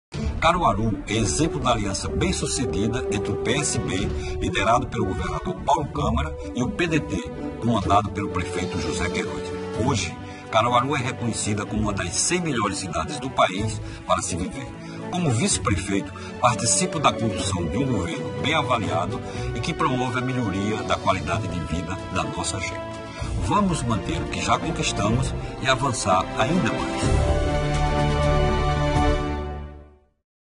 Inserção do PSB em Caruaru
Vice-prefeito de Caruaru, Jorge Gomes, participa de inserção do PSB no município.